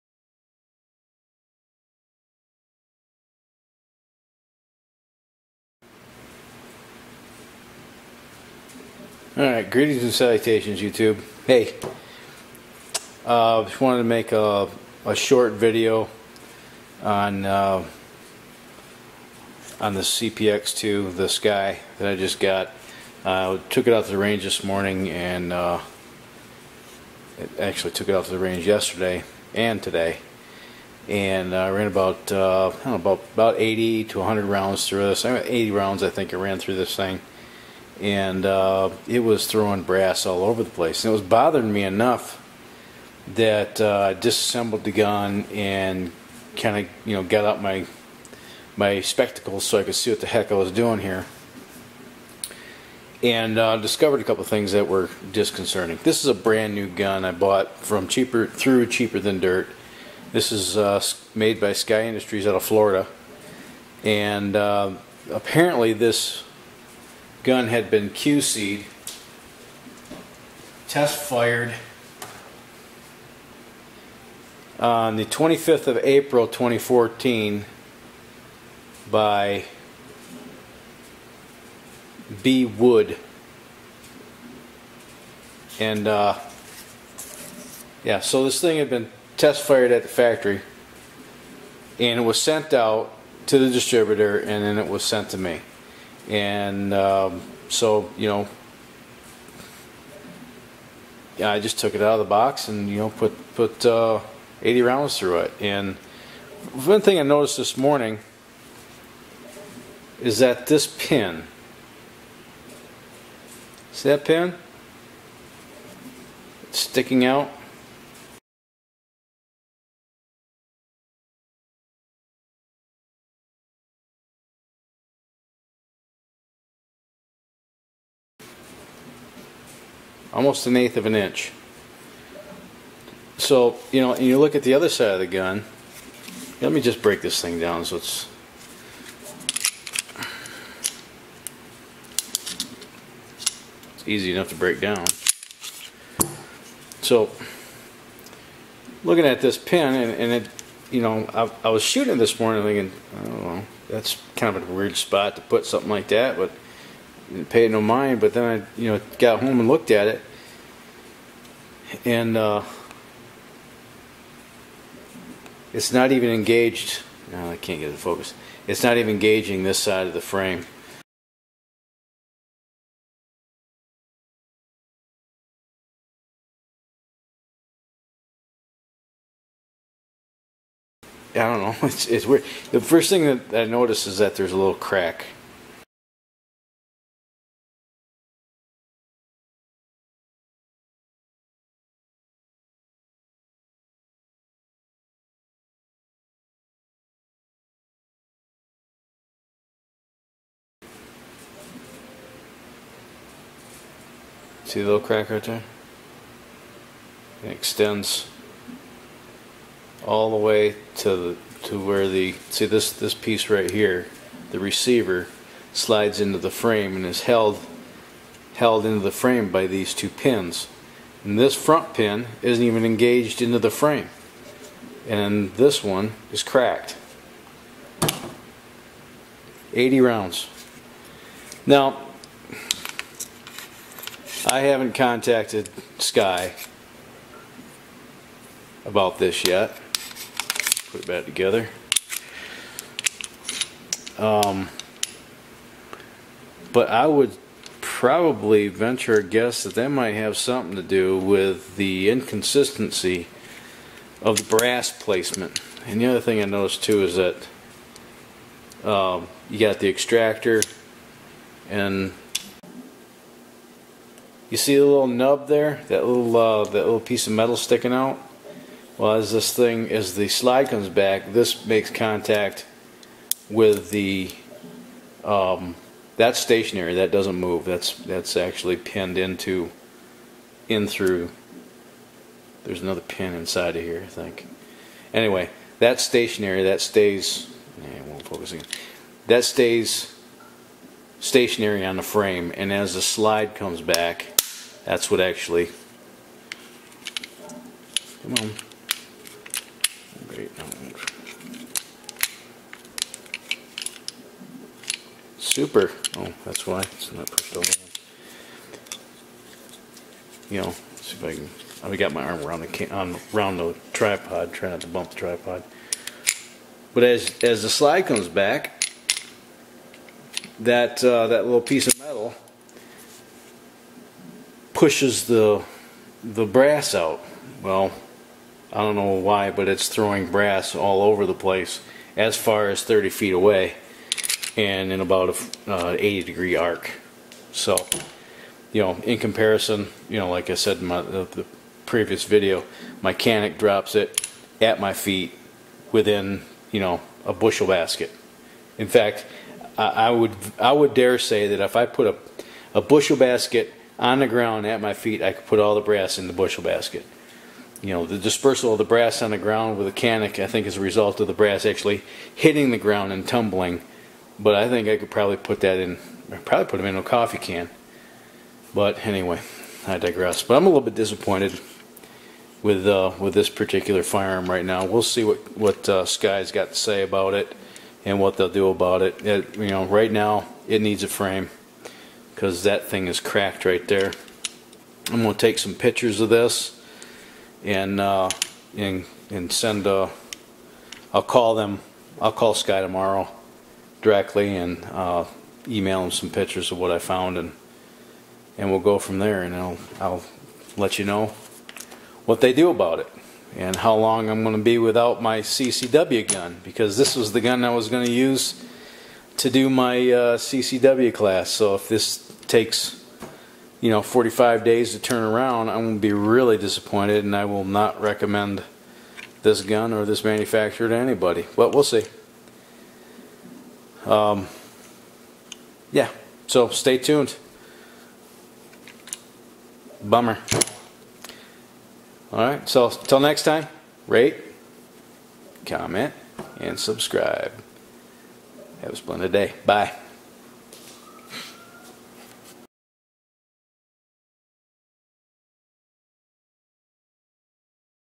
cpx 2 problems
Got home from the range, and I found some issues. mkdvojnqpfoivnoqerig-019834utpo134ngwrtin9248hg92